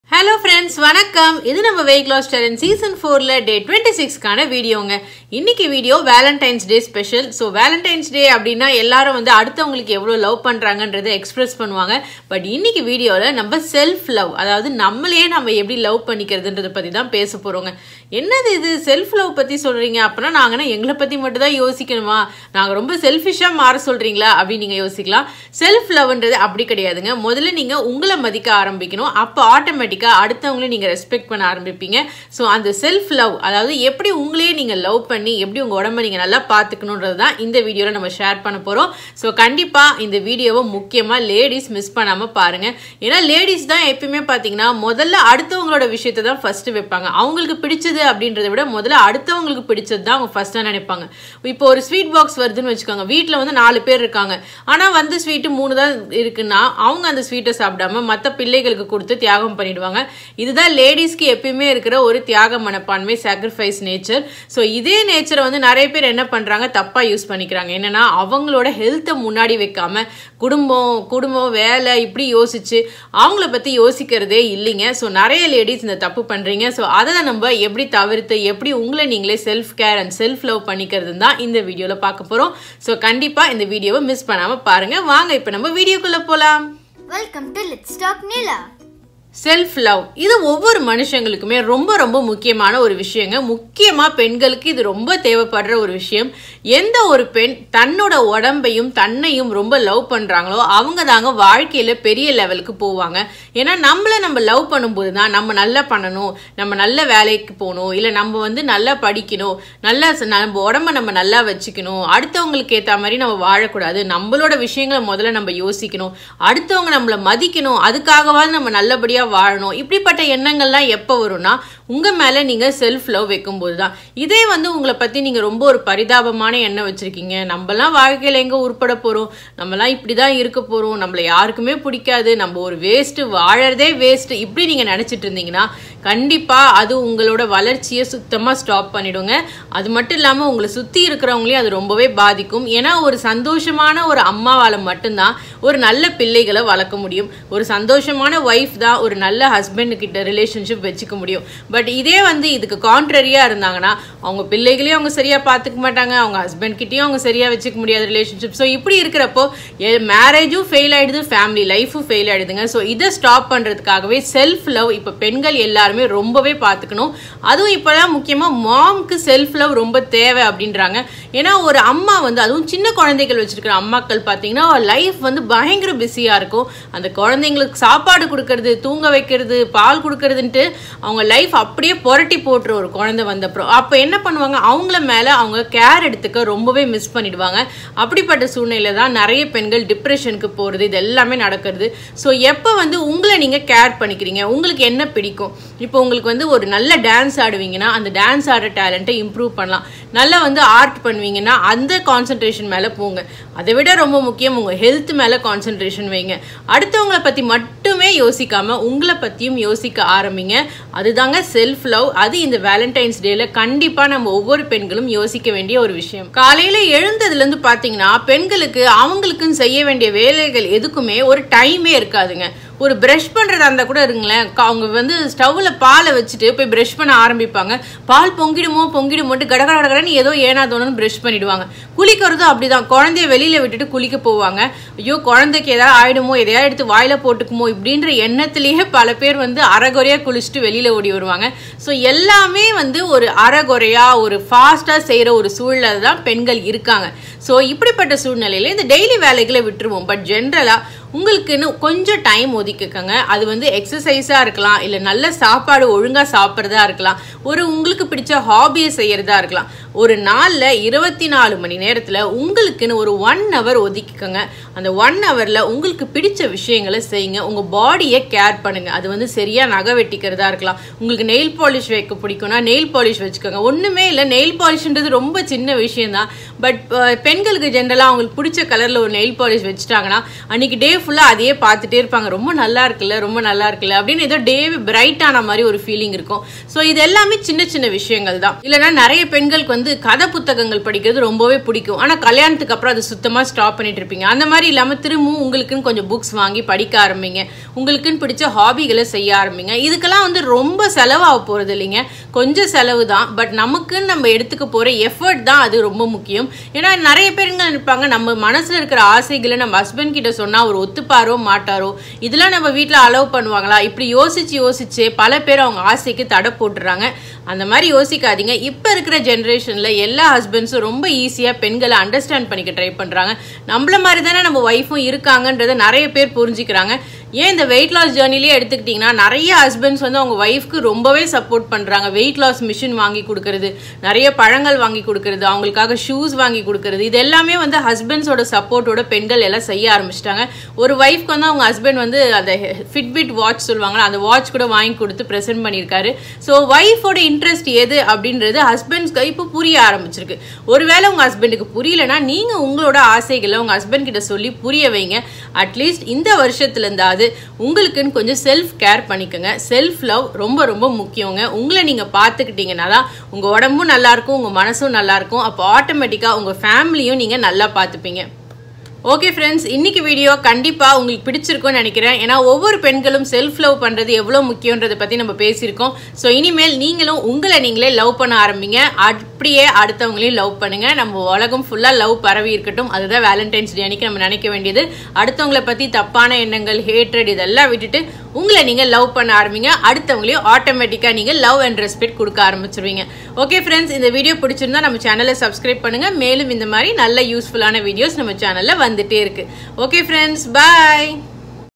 Day 26 Tamil Weight Loss Challenge | Self Love and Care | Women Take Care of Your Health Message
Hi dear ladies. This video I have shared how important is for women to take care of themselves first